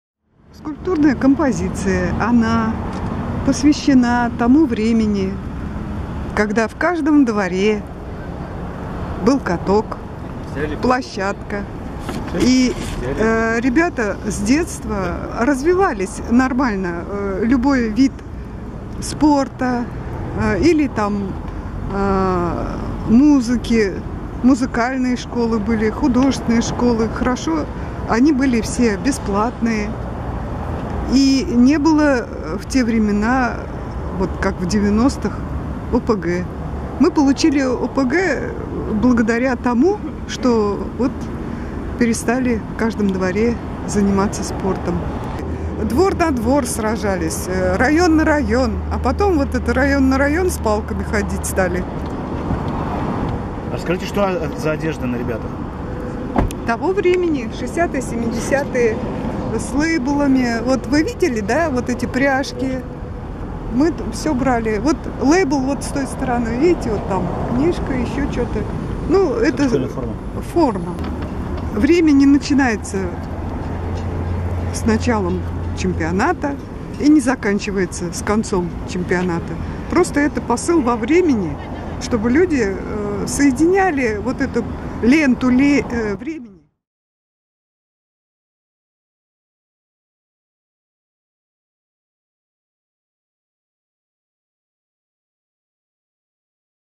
В Казани на улице Декабристов появились скульптуры школьников-футболистов
Сегодня в Казани на улице Декабристов установили новую скульптурную композицию — двух бронзовых советских школьников, которые играют в футбол после уроков. Об этом сообщает пресс-служба мэрии Казани.

Видео: Амир Сафин